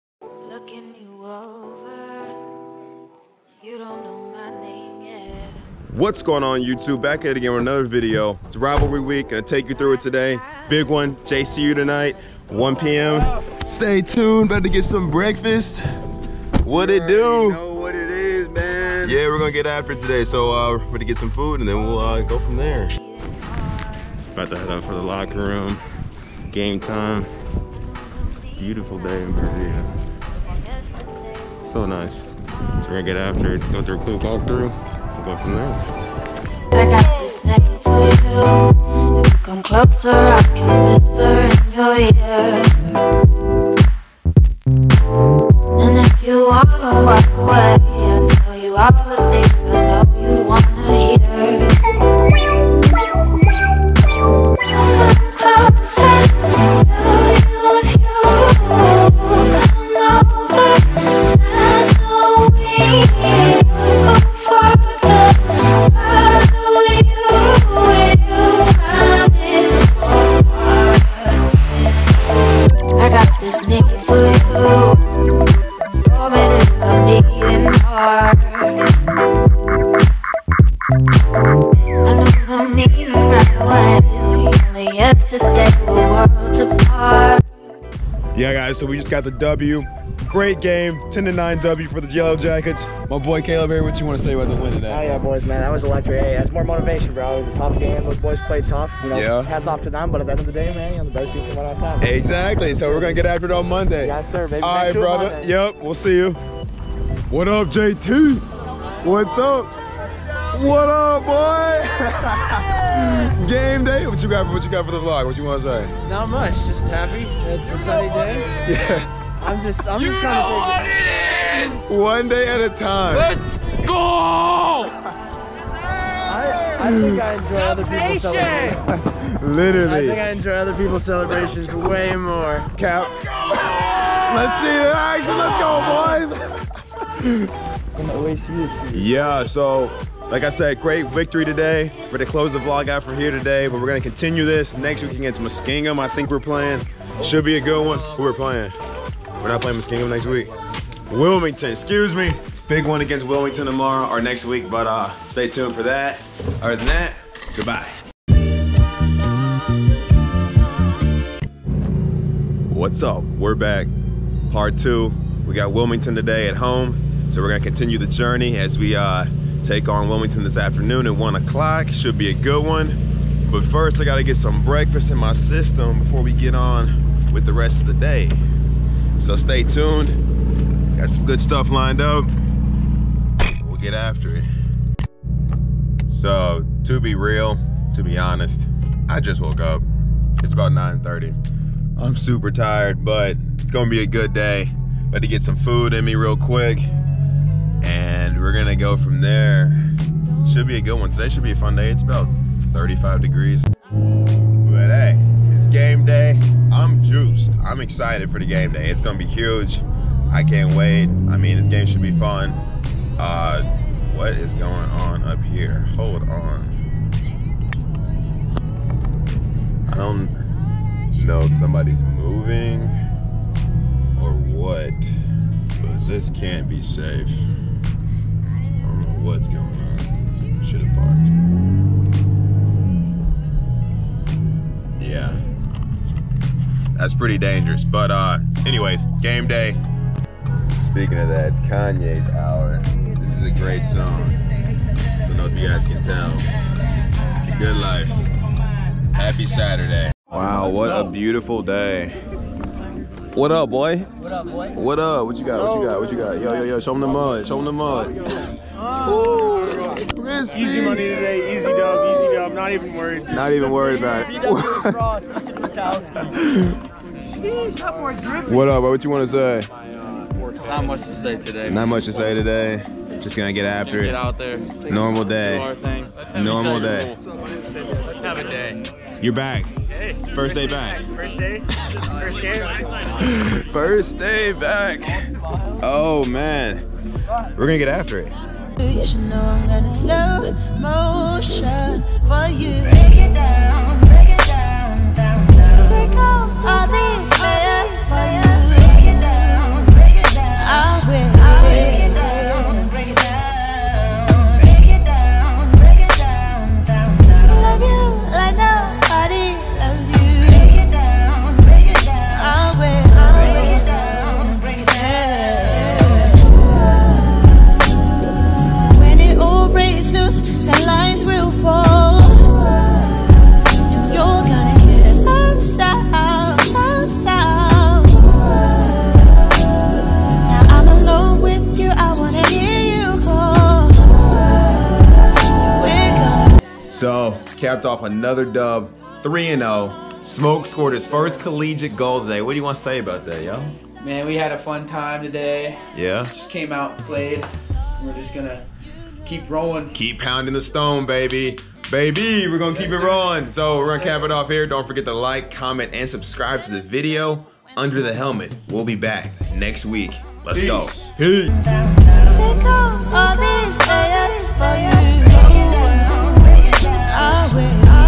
UNDER THE HELMET EP.2 : John Carroll & Wilmington (D3 Lacrosse) *VLOG*
Whats up guys, in todays episode I take you through our next two conference games versus crosstown rival John Carroll and Wilmington University! The boys got the job done.